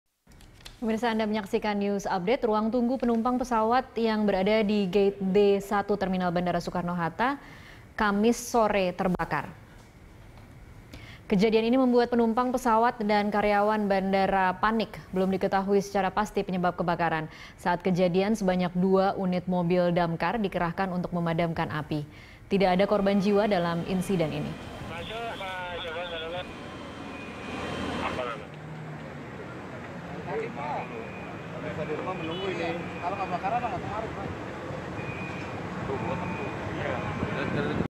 Kebakaran Landa Ruang Tunggu Penumpang di Bandara Soetta
Ruang tunggu penumpang pesawat yang berada di Gate D 1 Terminal Bandara Soekarno-Hatta terbakar pada Kamis (14/12/2017) sore.

Facebook.com/BeritaSatuTV
Youtube.com/BeritaSatu